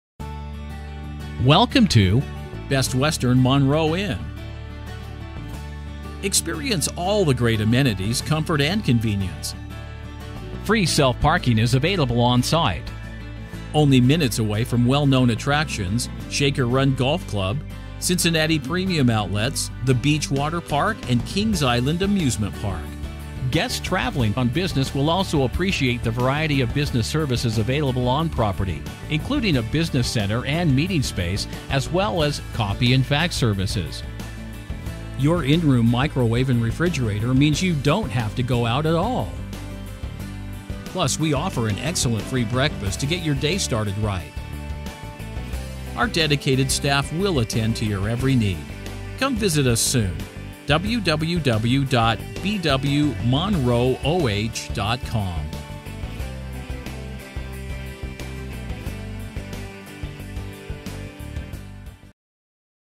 Best Western Monroe Inn Hotel in Monroe Ohio - Cincinnati Premium Outlets Hotels
The Best Western Monroe Inn is conveniently located off of I-75 in the city of Monroe OH. Only minutes away from well known Beach Waterpark and Kings Island Amusement Park. Get best deal for Cincinnati Premium Outlets Hotels